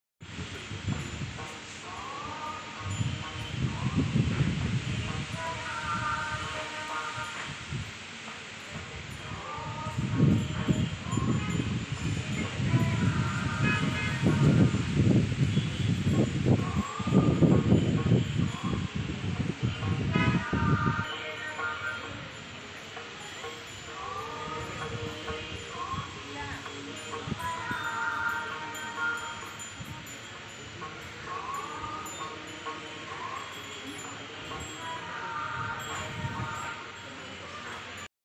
The Very Famous Temple || Talaipalem Shiva Temple
Talaipalem temple - Lord Shiva's temple
It is very famous temple at Andhra Pradesh . Very peaceful